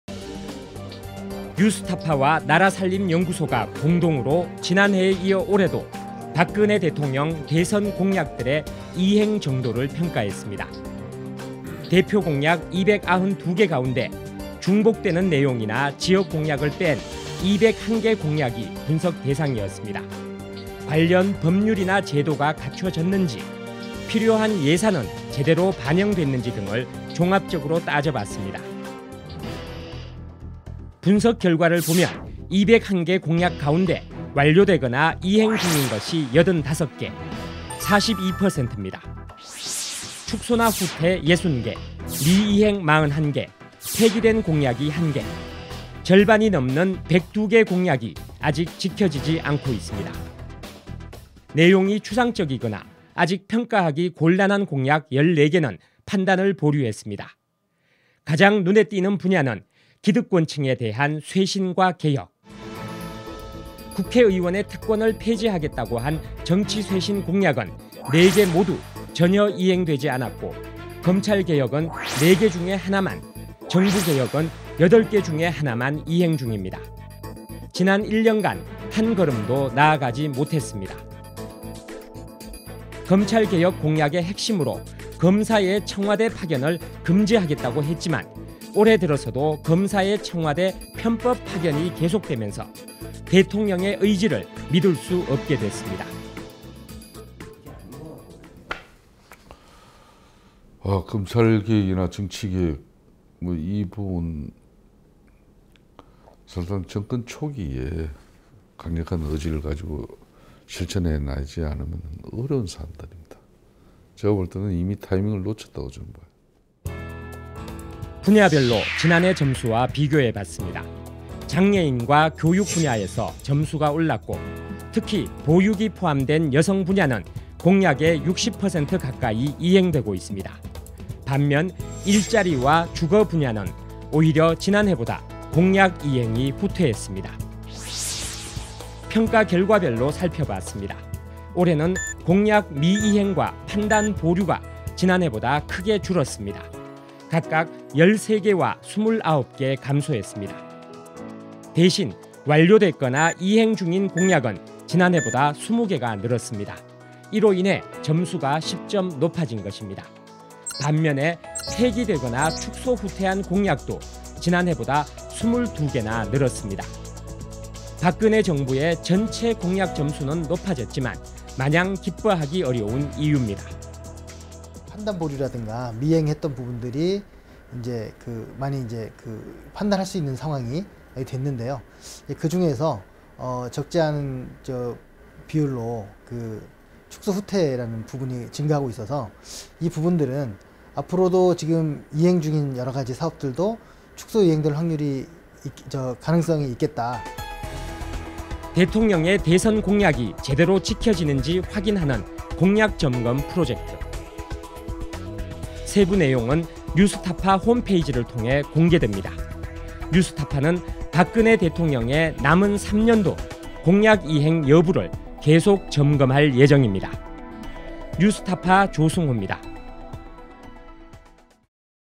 뉴스타파 - 박근혜 공약점수 10점 오른 ‘42점’ … 실상은?(2015.2.24)
박근혜 대통령의 대선 공약 이행 점수는 42점입니다. 지난해보다 10점이 올랐지만, 실상을 들여다보니 기뻐할 수 만은 없었습니다.